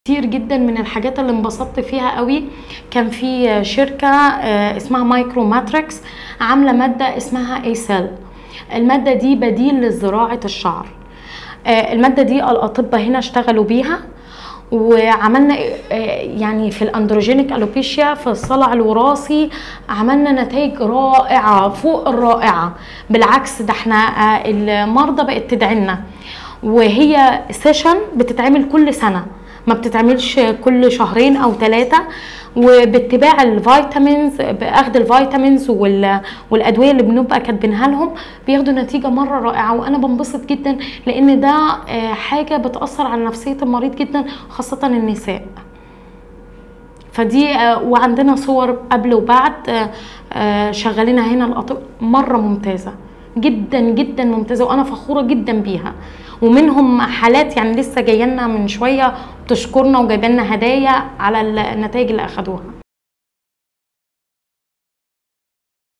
بدائل زراعة الشعر: حلول طبيعية وتقنية لتساقط الشعر
إعداد: فاطمة مصطفى
تصوير : نادر نبيل